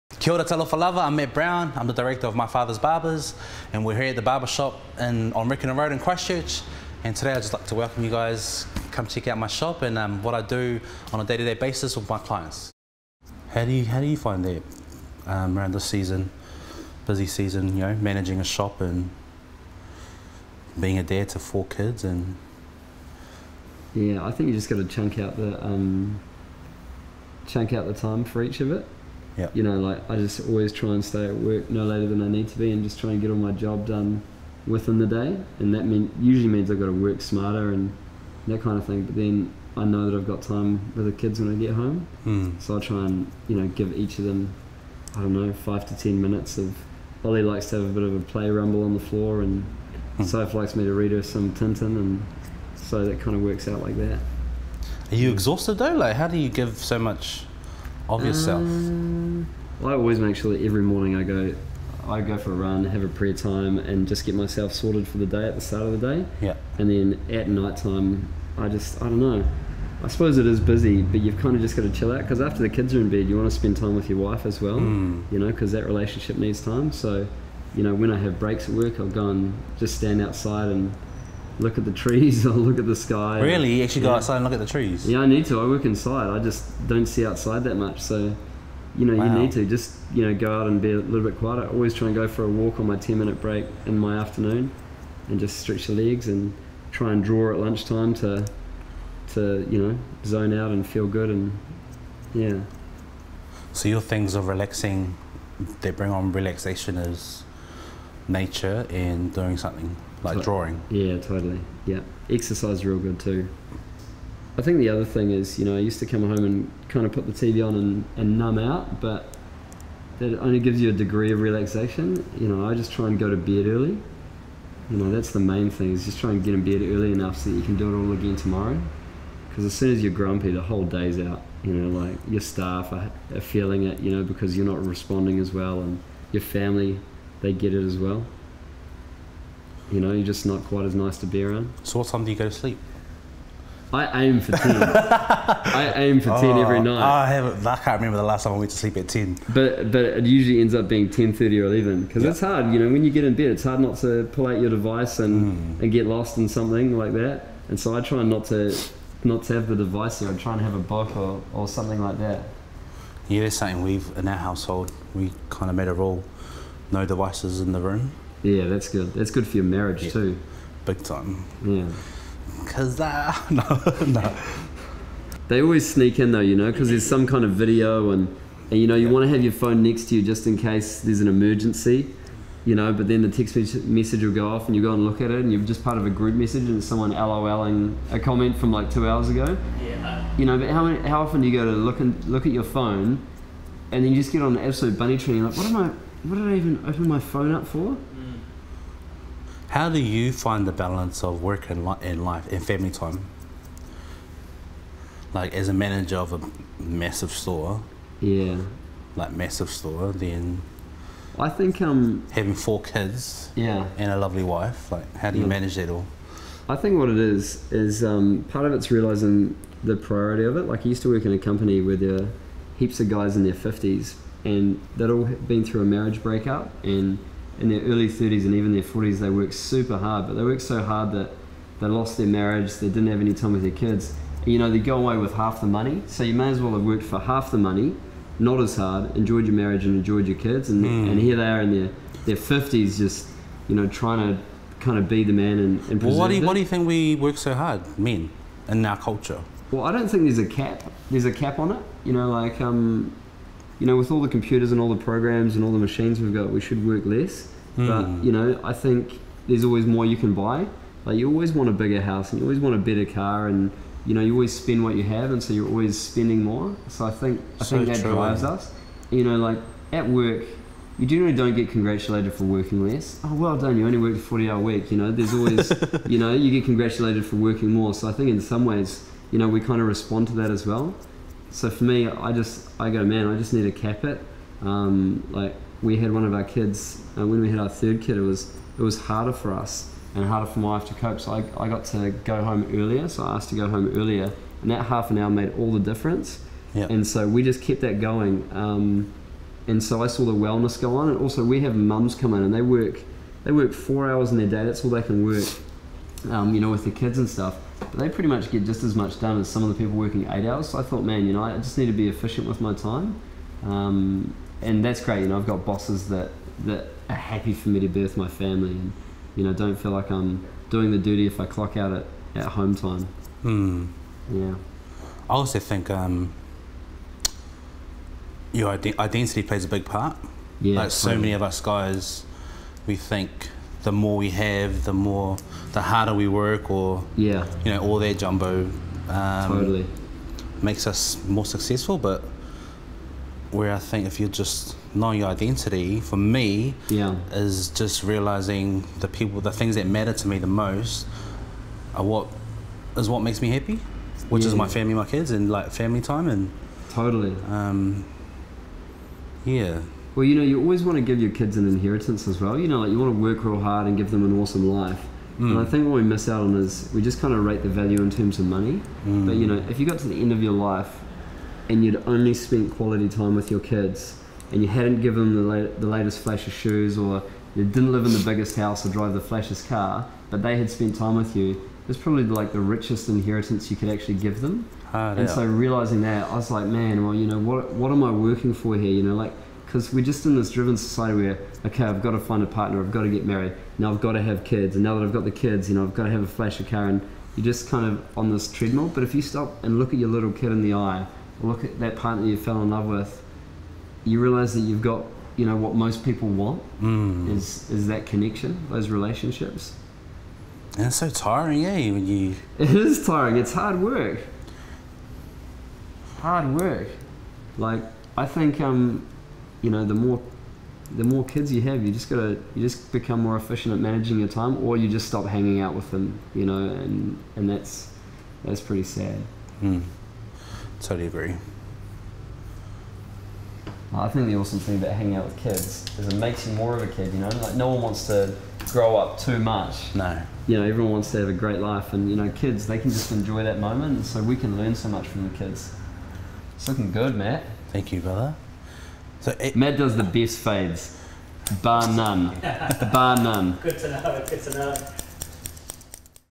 My Fathers Barbers - What is means to be a kiwi man.
Nothing is out of bounds in Matt's chair at My Fathers Barbers. It's where his clients and friends come to chat about what going on in their lives, their problems, and their successes. We think that's Manly As.